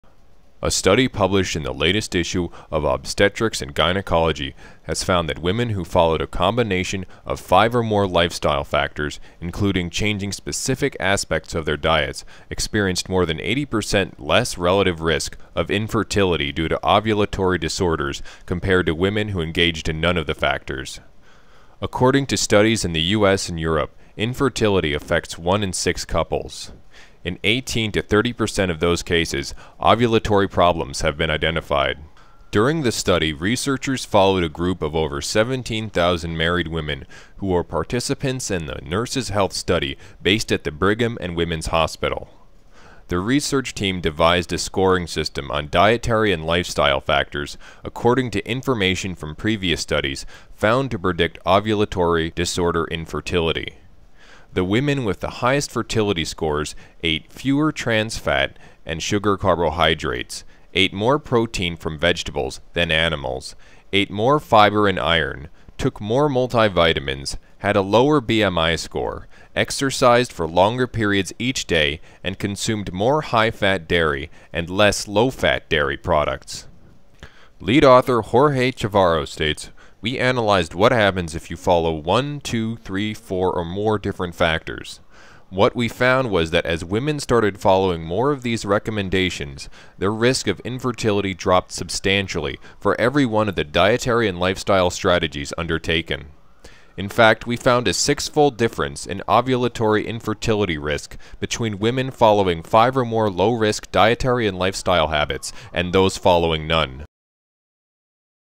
Empowered Health News | Lifestyle change prevent Infertility
A study published in the latest issue of Obstectrics and Gynecology has found that women who followed a combination of five or more lifestyle factors, including changing specific aspects of their diets, experienced more than 80 percent less relative risk of infertility due to ovulatory disorders compared to women who engaged in none of the factors. According to studies in the U.S. and Europe, infertility affects one in six couples. In 18 to 30 percent of those cases, ovulatory problems have been identified.

Story is produced and provided by Empowered Medical Media, LLC
to see the full story